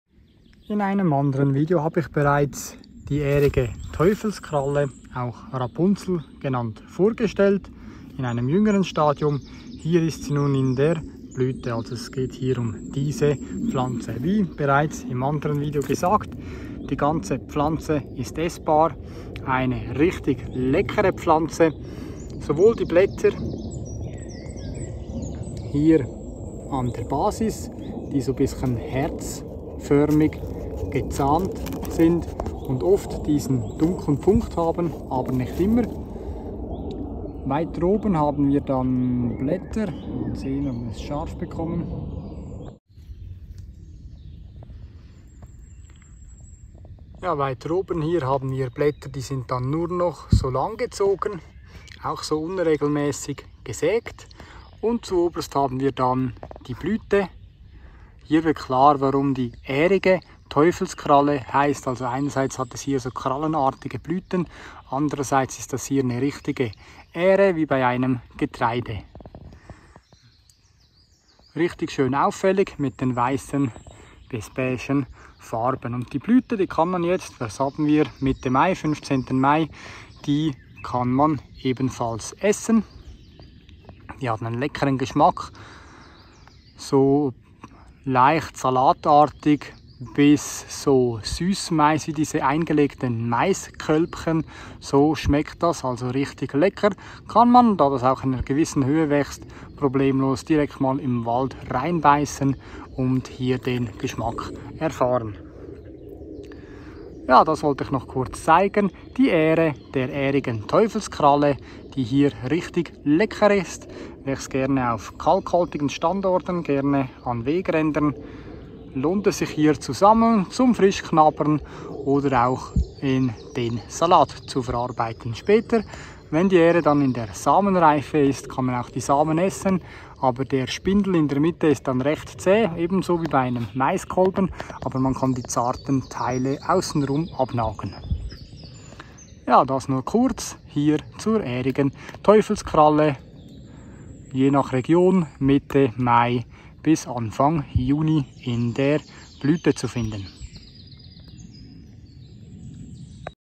Die Ähre der Ährigen Teufelskralle
Wie eine Mischung aus Süssmais und Salat, so ähnlich schmeckt die Ähre der Ährigen Teufelskralle.

Hier gehts zum ganzen Porträt
Das wahrscheinlich beste heimische Wildgemüse - Die Ährige Teufelskralle